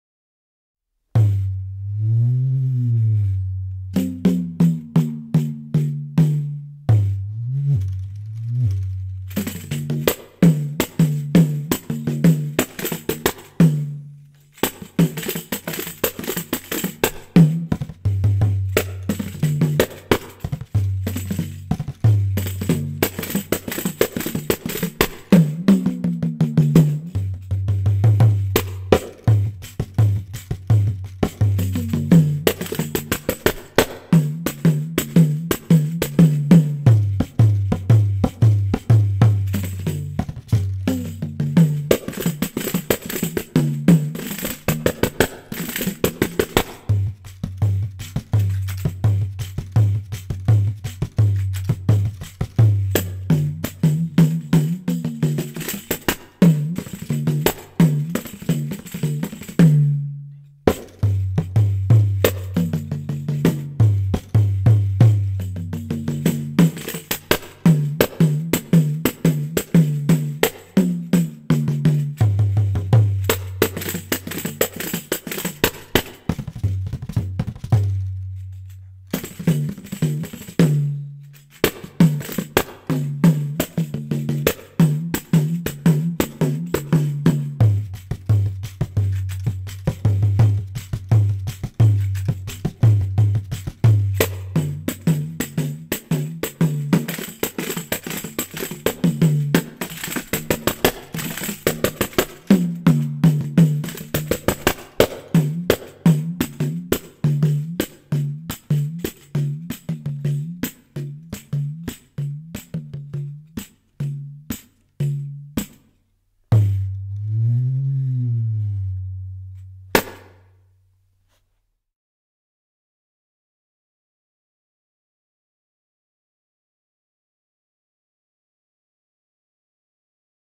Solo for @GonBopsTube
This is another seculsion solo featuring two pandeiros (8" and 10"). I'm using some traditional pandeiro strokes as well as some Indian split hand technique and some borrowed strokes from South Italian tamburello strokes. Please enjoy, post and share as you like.
Stay well,
-Tom